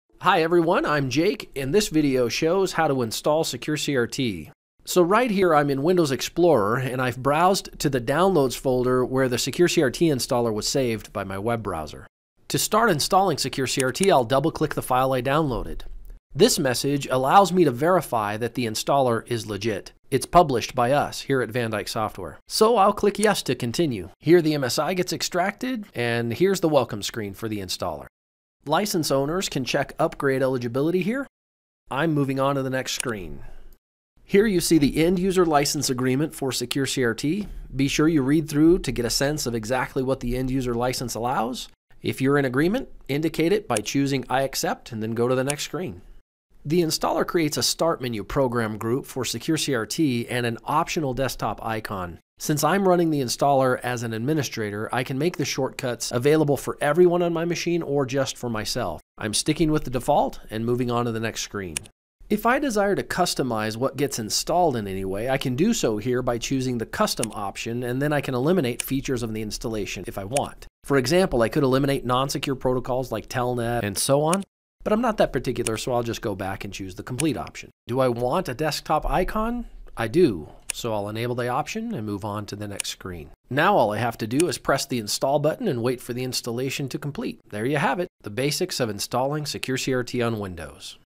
How to Install SecureCRT
Provides a walk-through showing how to install SecureCRT on Windows.